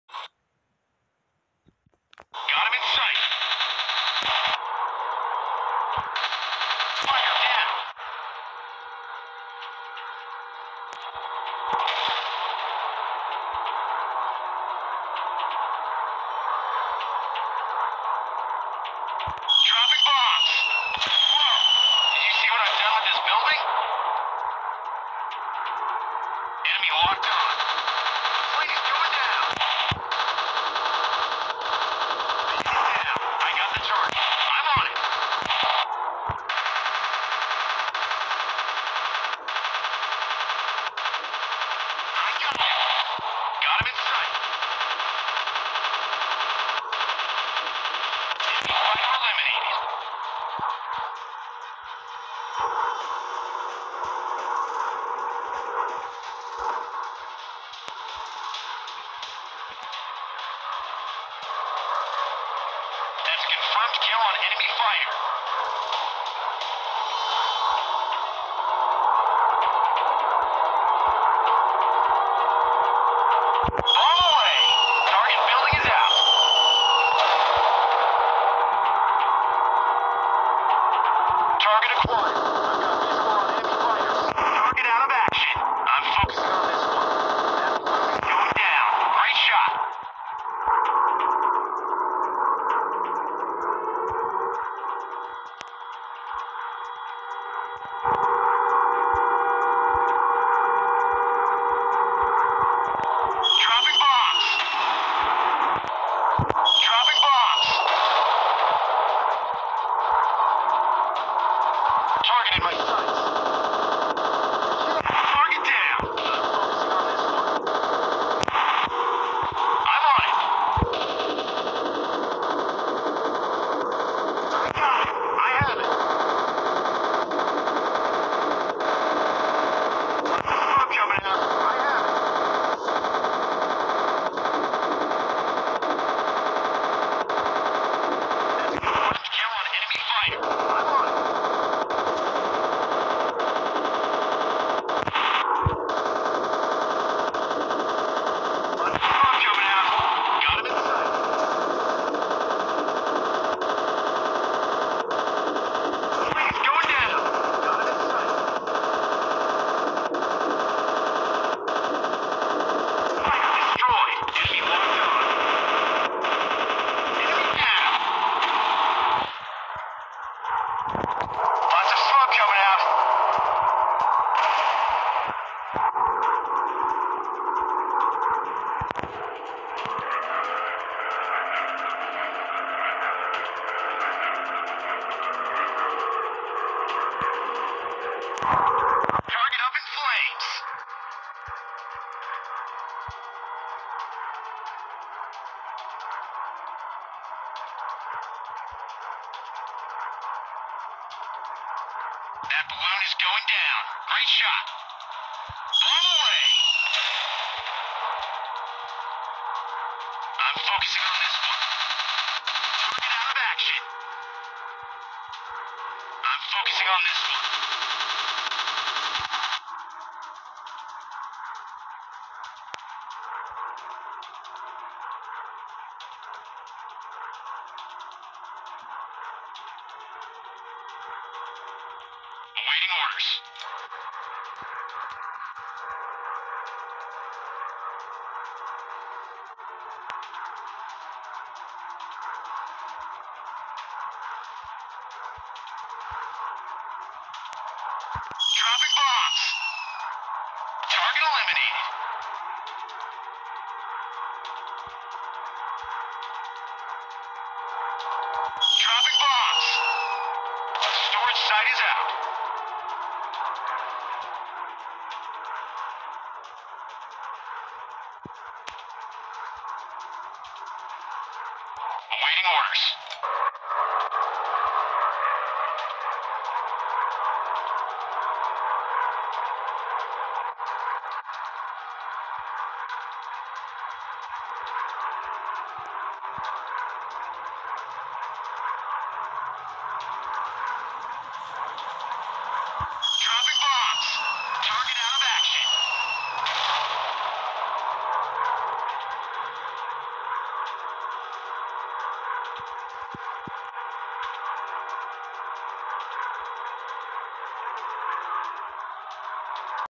The most beautiful and easiest warplanes game to play / creative commons
Welcome to the most beautiful and fun warplane game in the world. this game are very realistic.  You will play this game without getting tired. Let's play this game together.
creative commons